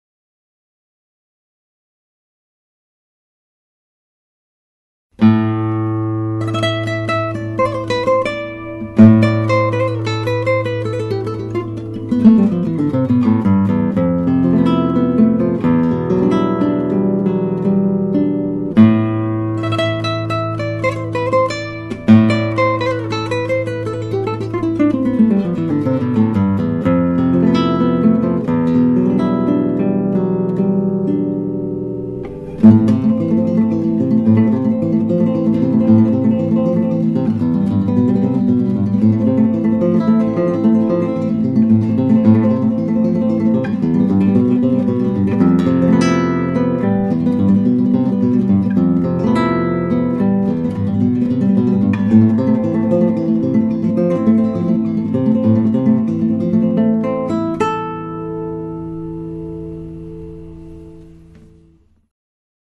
1969 Ramirez 1a Brazilian/Cedar at Dream Guitars
As soon as you play the first note on this guitar, you can tell it's been played a lot. It's alive and powerful with all of the clarity and balance you would expect from a 1960's Ramirez. Ramirez fans know that the 1960s model were special guitars. This one is a great example, it's very alive and rich sounding. The recent setup was done well and this one plays like a new guitar. This is a guitar for the player who cares only about tone and wants the best.